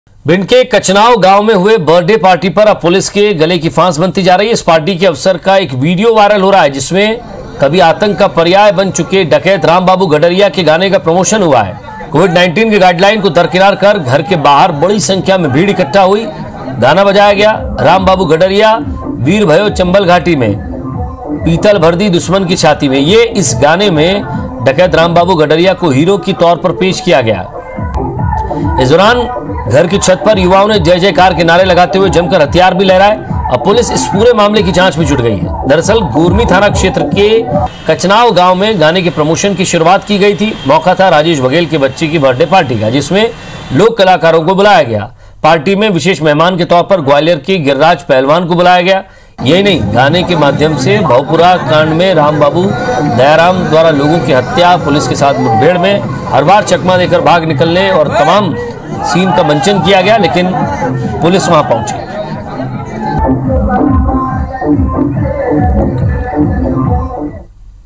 Bhind में डकैत रामबाबू गडरिया के गाने का प्रमोशन | Birthday Party में लहराए गए हथियार
IBC24, Bhind में डकैत रामबाबू गडरिया के गाने का प्रमोशन, Birthday Party में लहराए गए हथियार